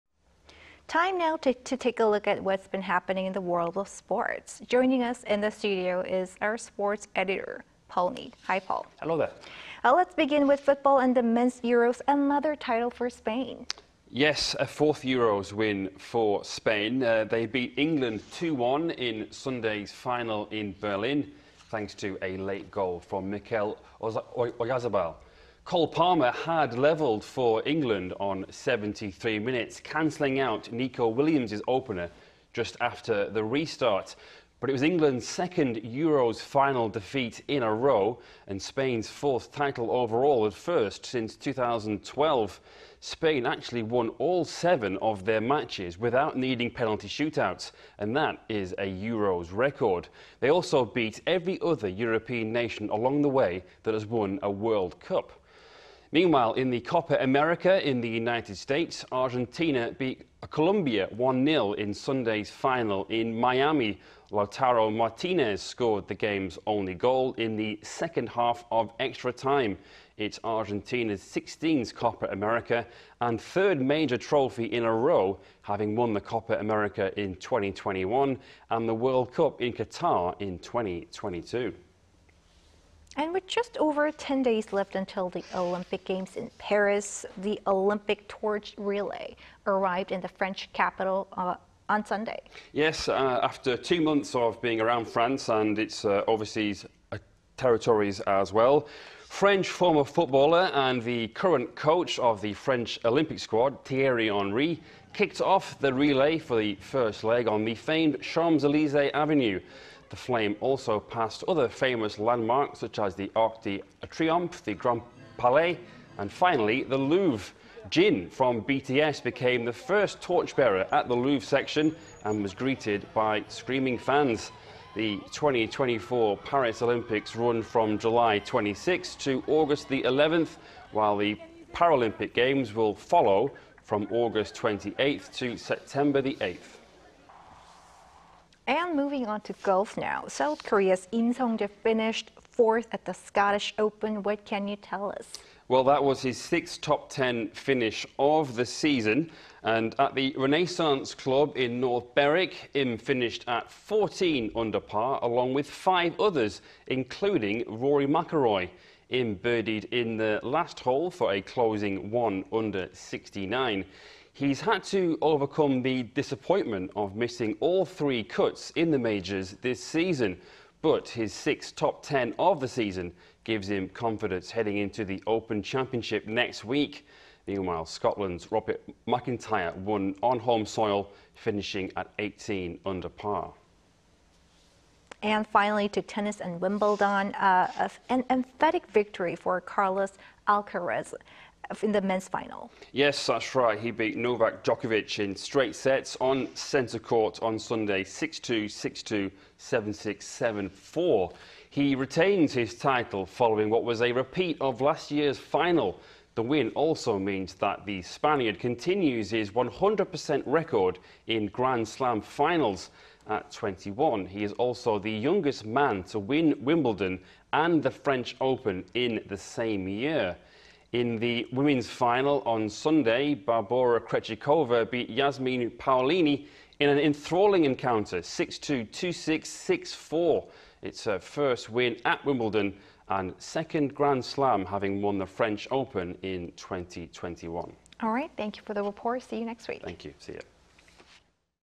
[Sports Round-up] Spain win Euro 2024, Argentina win Copa America
스페인 유로 우승, 아르헨티나 코파 아메리카 우승

Time now to take a look at what's been happening in the world of sports.
Joining us in the studio is our sports editor Paul Neat.
Hello, Paul.

Hello.

Let's begin with football and the men's Euros.
Another title for Spain.

Yes, a fourth Euros win for Spain.
They beat England 2-1 in Sunday's final in Berlin thanks to a late goal from Mikel Oyarzabal.
Cole Palmer had leveled for England on 73 minutes, canceling out Nico Williams' opener just after the restart.
It was England's second Euro's final defeat in a row, but Spain's fourth title and first since 2012.
Spain actually won all seven of their matches without needing penalties and that is a Euros record.
They also beat every other European nation along the way that has won a World Cup.
Meanwhile, in the Copa America in the United States, Argentina beat Colombia 1-0 in Sunday's final in Miami.
Lautaro Martinez scored the game's only goal in the second half of extra time.
It's Argentina's 16th Copa America and third major trophy in a row having won the Copa America in 2021 and the World Cup in Qatar in 2022.

With just over 10 days left until the Olympic Games in Paris, the Olympic Torch Relay arrived in the French capital on Sunday.

Yes, after around two months of traveling through France and its overseas territories.
French former footballer and the current coach of the French Olympic squad, Thierry Henry, kicked off the relay for the first leg on the famed Champs-Élysées Avenue.
The flame also passed landmarks such as the Arc de Triomphe, the Grand Palais, and finally the Louvre.
Jin from BTS became the first torchbearer at the Louvre section and was greeted by screaming fans.
The 2024 Paris Olympics run from July 26th to August 11th, while the Paralympic Games will follow from August 28th to September 8th.

Moving on to golf now.
South Korea's Im Sung-jae finished fourth at the Scottish Open.
What can you tell us?

Yes, that's his sixth top-ten finish of the season.
At The Renaissance Club in North Berwick, Im finished at 14-under par along with five others, including Rory McIlroy.
Im birdied in the last hole for a closing 1-under 69.
He's had to overcome the disappointment of missing all three cuts in the majors this season.
But, his sixth top-ten of the season gives him confidence heading into The Open Championship next week.
Meanwhile, Scotland's Robert MacIntyre won on home soil, finishing at 18 under par.

And finally, to tennis and Wimbledon.
An emphatic victory for Carlos Alcaraz in the men's final.

Yes, Carlos Alcaraz beat Novak Djokovic in straight sets on Centre Court on Sunday , 6-2, 6-2, 7-6 -7-4.
He retains his title following what was a repeat of last year's final.
The win also means that the Spaniard continues his 100-percent record in Grand Slam finals.
At 21, he is also the youngest man to win Wimbledon and the French Open in the same year.
In the women's final on Saturday, Barbora Krejcikova beat Jasmine Paolini in an enthralling encounter. 6-2, 2-6, 6-4.
It's her first win at Wimbledon and second Grand Slam having won the French Open in 2021.

Alright, thanks for that, Paul.

Thanks.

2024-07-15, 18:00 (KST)